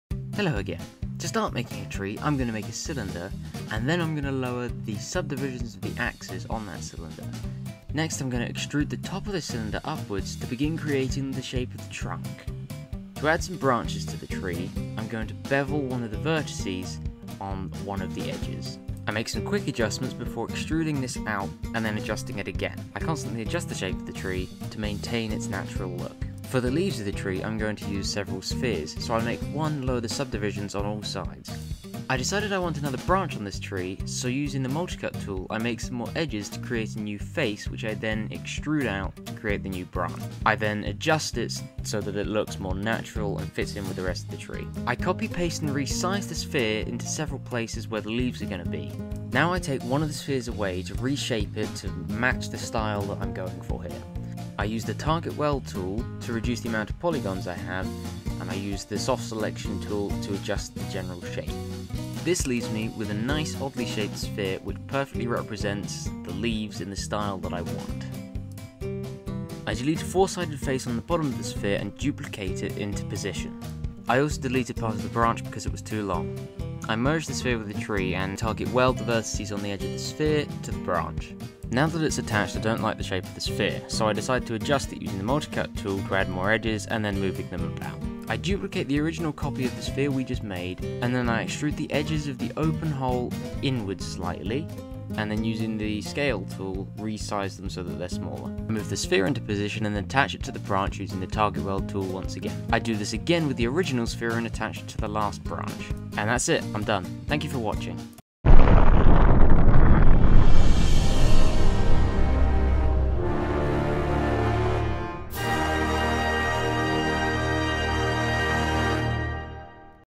Low Poly Tree | Quick Maya Tutorial
►Another maya guide for you all! This time we look at my method for how to 3D model a low poly tree! The next one will go live next Friday at 3PM BST! Leave any feedback or questions you might have in the comment section below!